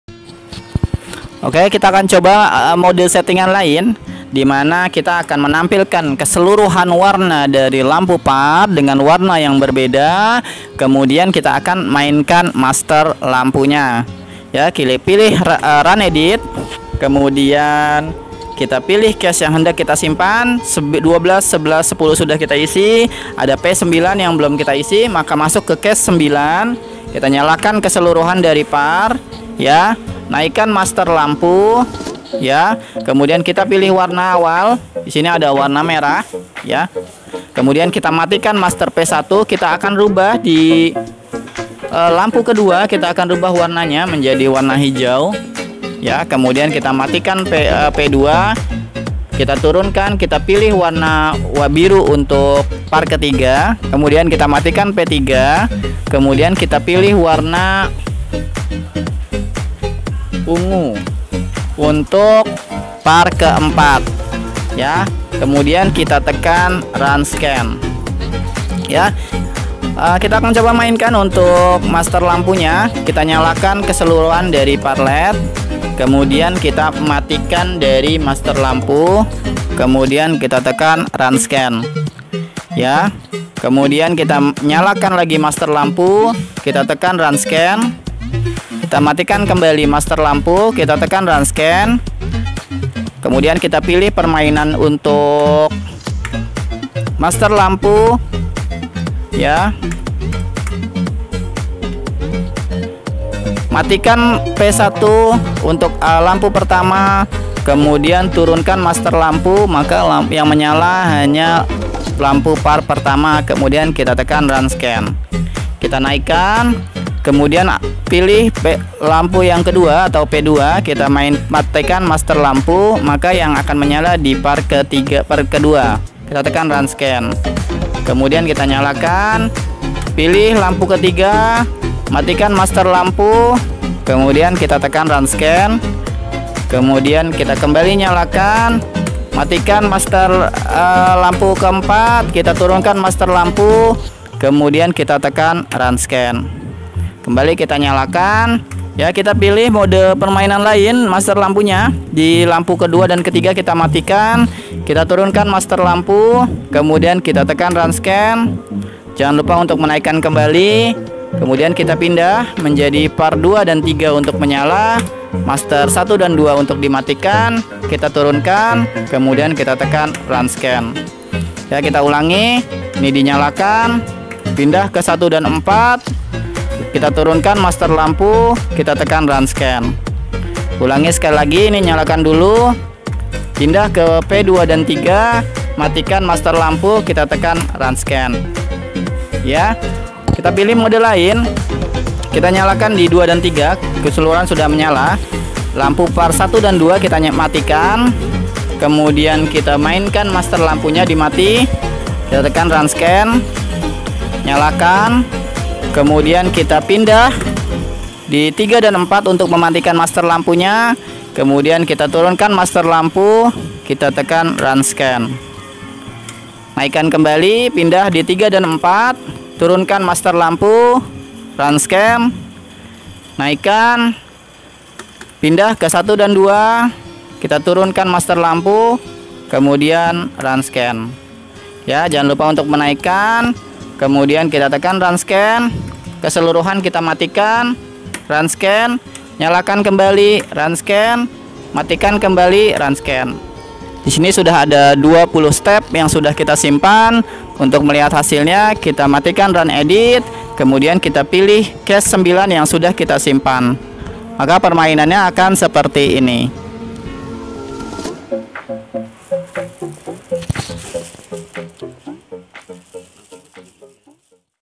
Setting Par LED Lanjutan DMX 240 - Prima Jaya LED
DMX 512 mixer 240 Disco merupakan controler lighting favorite medium stage dengan model 16 Channel dan 12 Chase penyimpanan.

Terdapat 2 Output DMX 512 Untuk cabang Lampu dan pastinya Mudah dalam pengoperasian.

Produk Berkualitas dengan Jaminan Garansi 1 Tahun
Info Harga & pemesanan